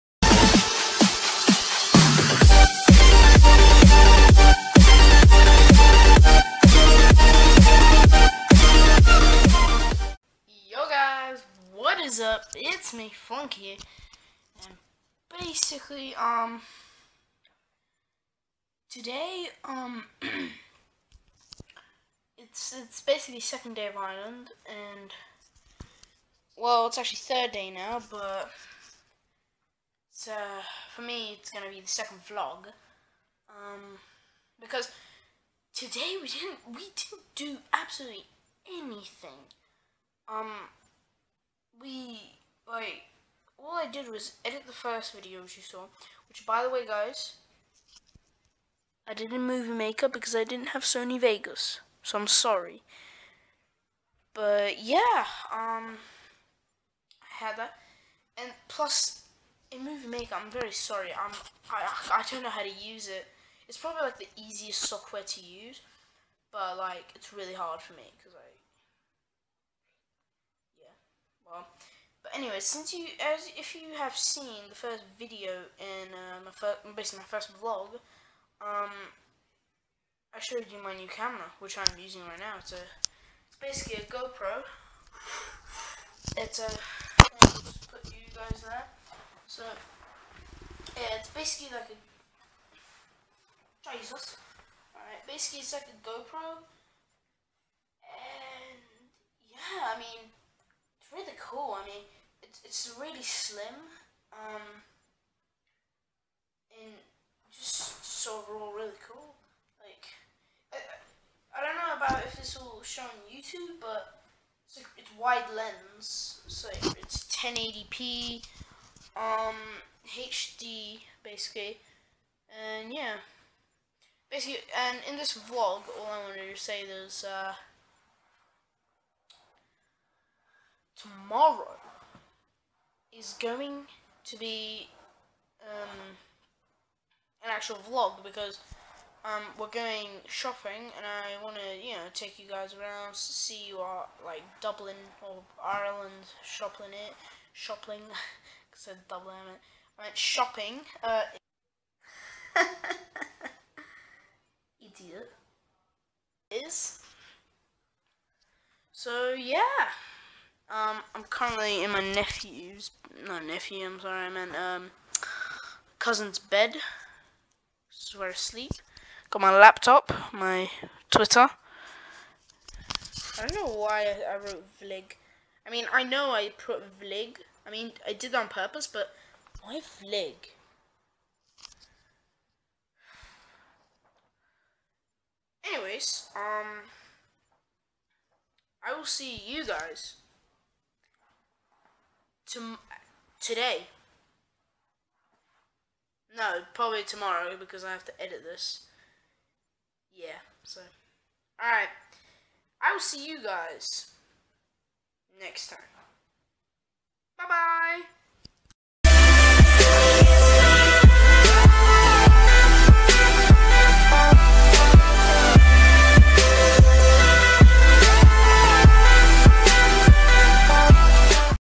Flunky's Archive: Vlog 0002
Yes guys I'm back with another Vlog cuz y not...and sorry for it being very short there was literally nothing to show that day because we just stayed in that day because of the hangover from the party but I was just showing off my new camera and it's so k3w1 m69 anyways bye bye

NOTE: THIS WAS DONE IN MOVIE MAKER BECAUSE I DIDN'T HAVE SONY VEGAS ON MY LAPTOP SOZ GUYS :'(